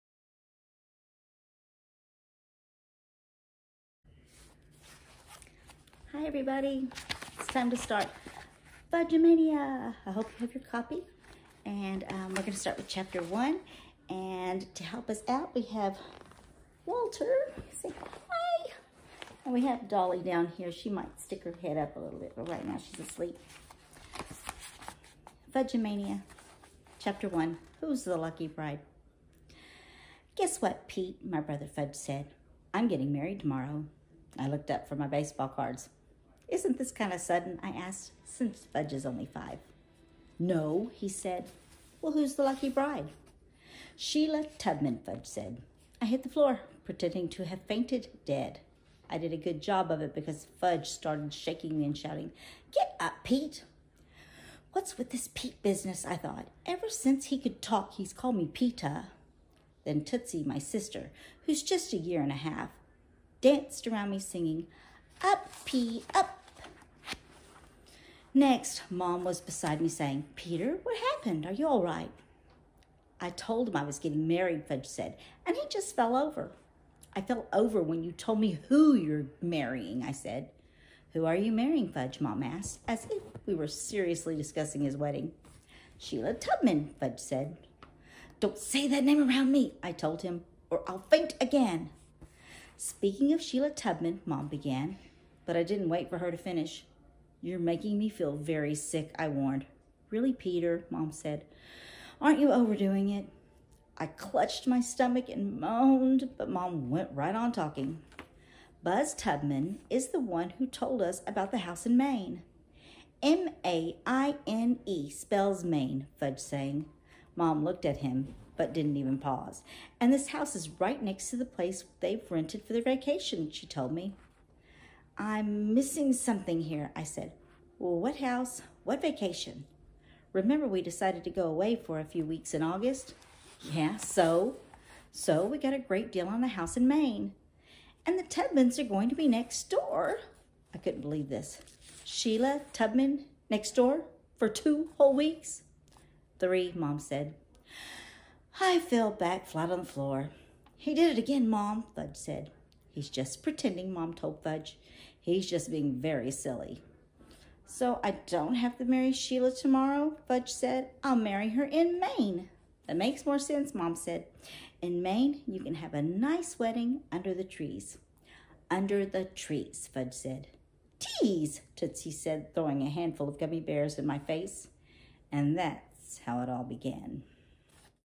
FUDGE-A-MANIA: Chapter 1 Who's the Lucky Bride?
Athens Reads Together fall 2020 selection FUDGE-A-MANIA by Judy Blume